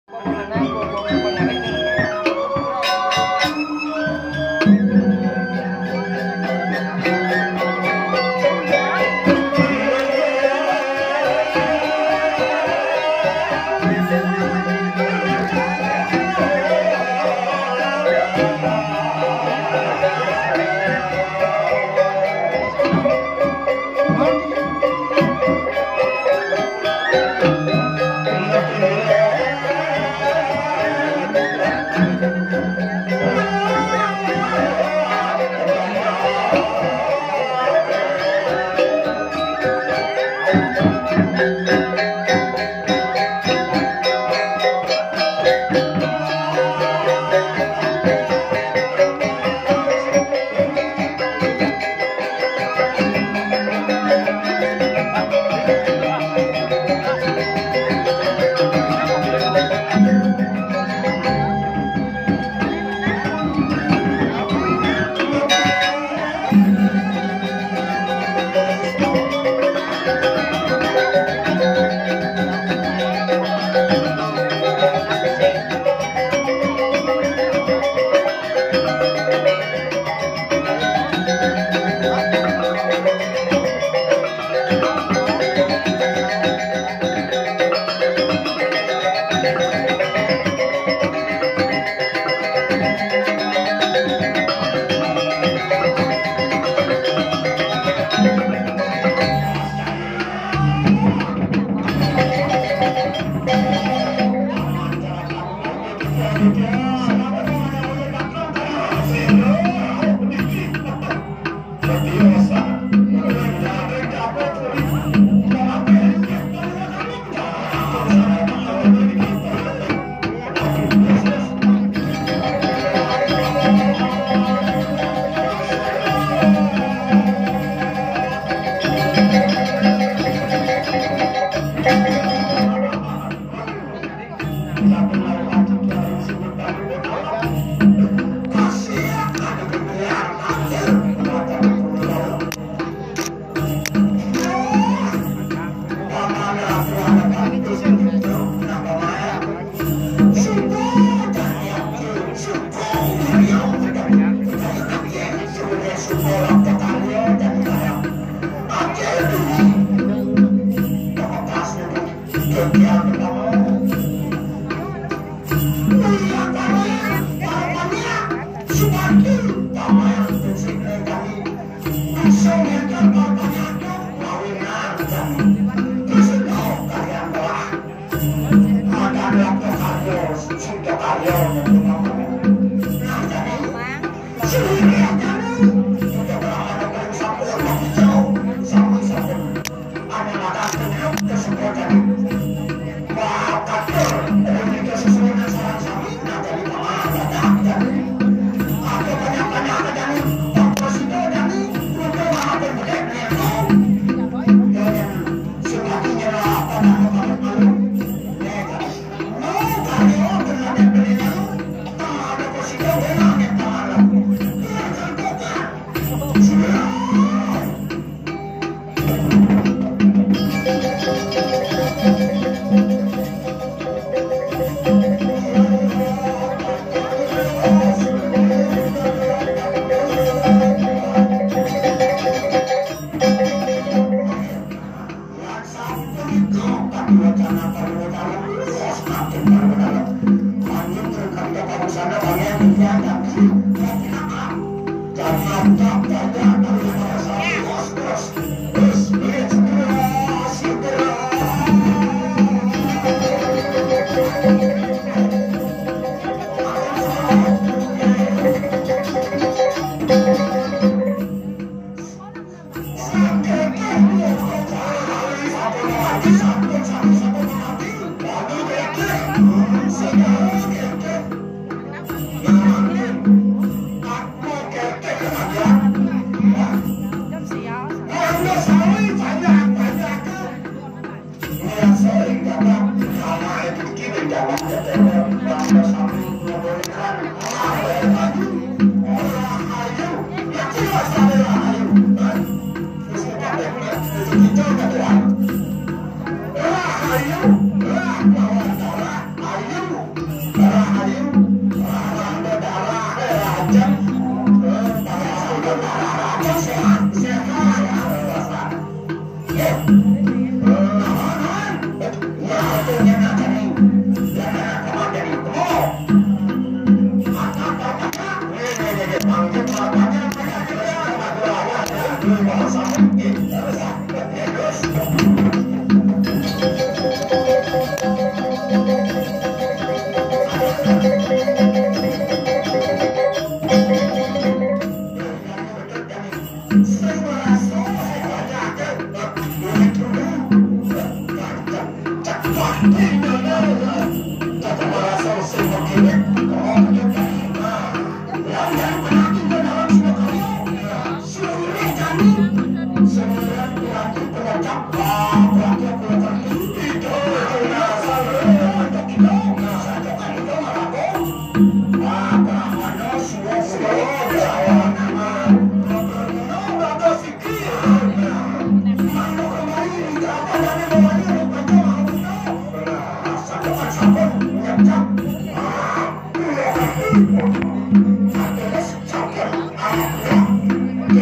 Topeng Sang Hyang Dedari Pura Dalem Ketewel Sukawati #2
Tarian ini di pentaskan saat Upacara Besar di Pura Dalem Ketewel Sukawati Gianyar